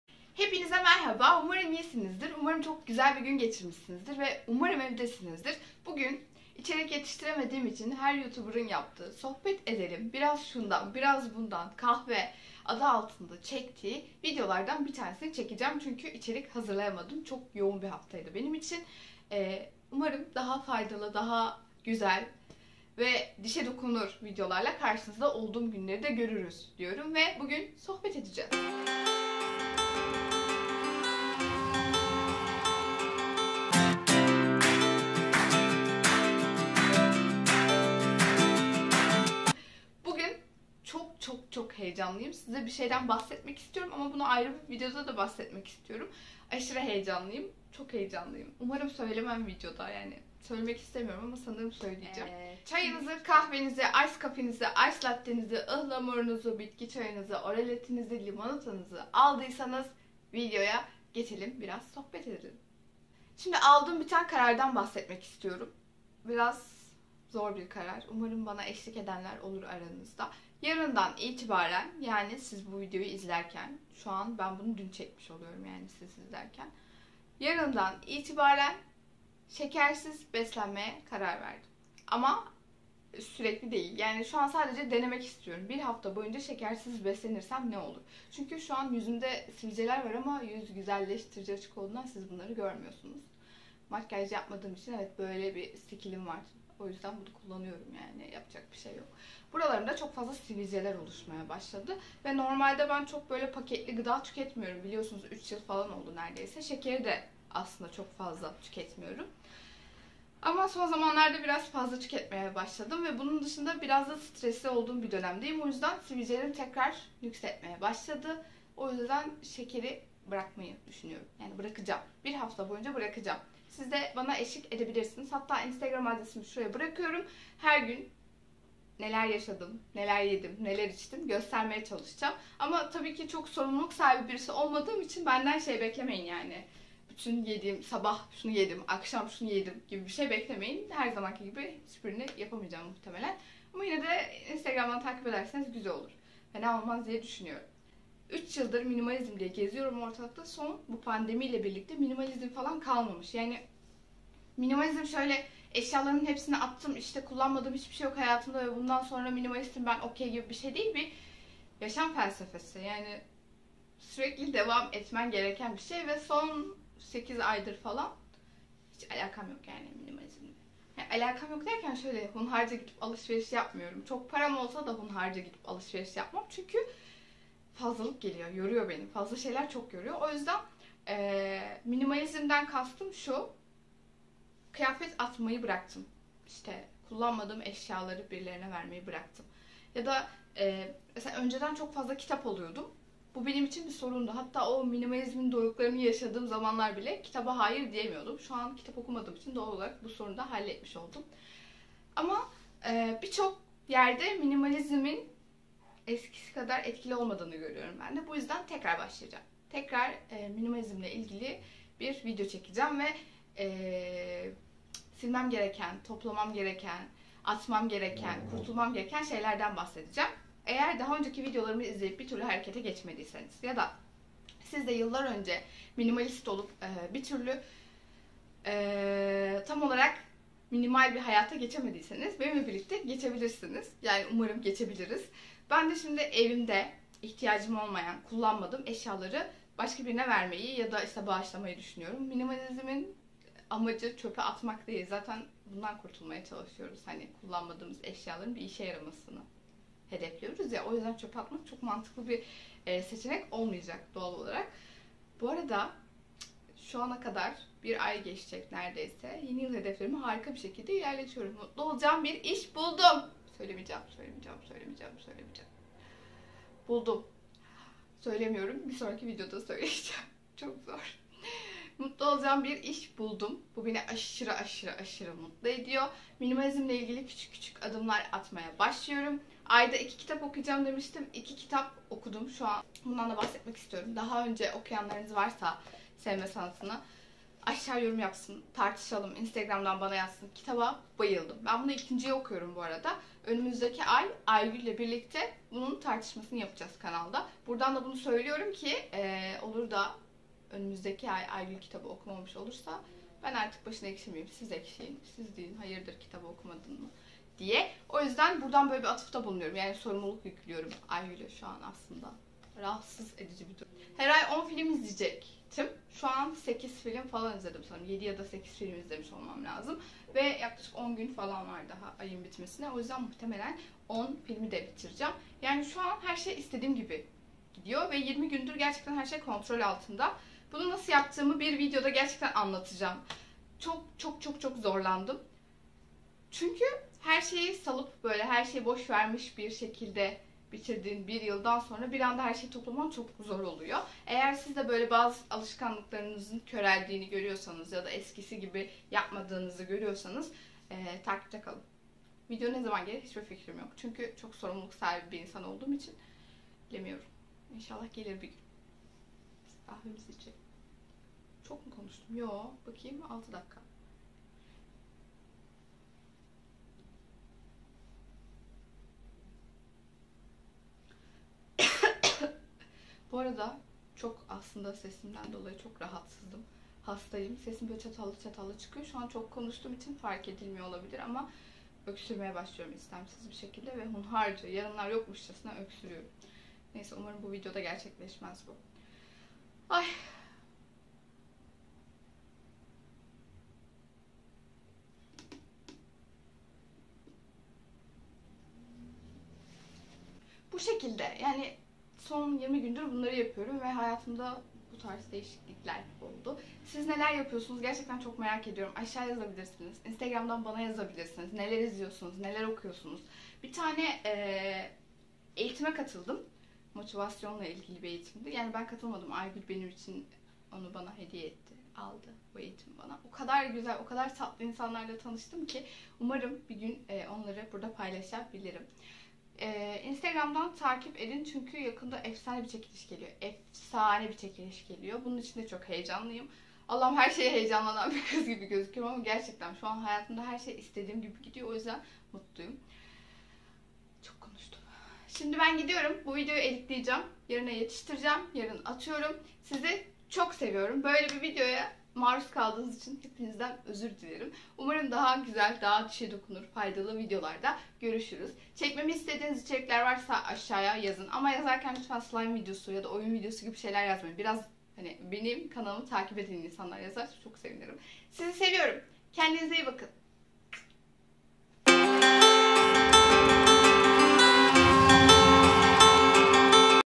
kahvenizi alın sohbet edelim / şekersiz beslenme / minimalizm / boş yaptım yine
Video yetiştiremediğim için boş yaptım üzgünüm ama çok değil.